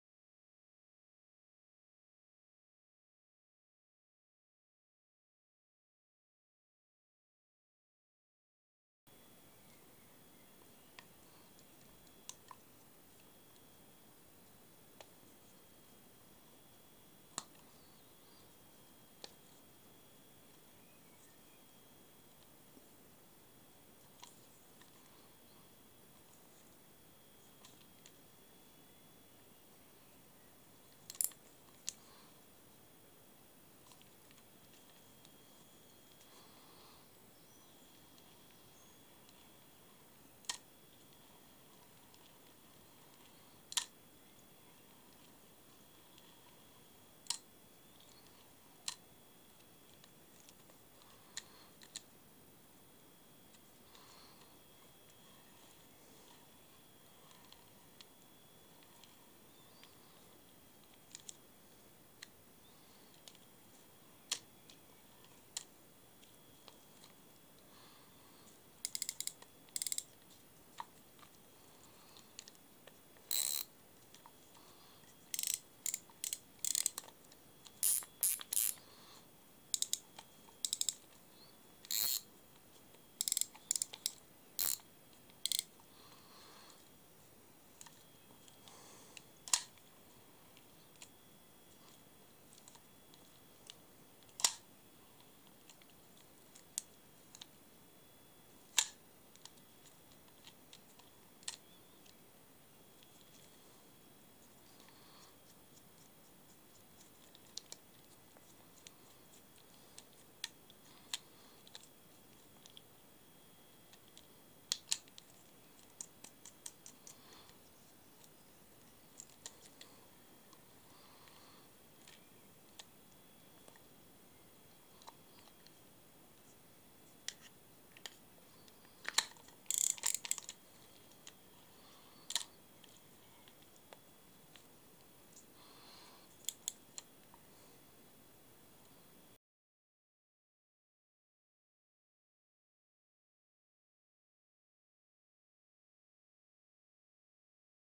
Okuma Undertaker UR50 - Carp & Coarse Fishing Rear Drag Fixed Spool Spining Reel
Okuma Undertaker UR50 - Rear Drag Fixed Spool Fishing Reel.
An excellent if dubiously named size 5 reel from Okuma suitable for heavy coarse, carp and specimen fishing. The handle on this one is outstandingly good. A darn good reel.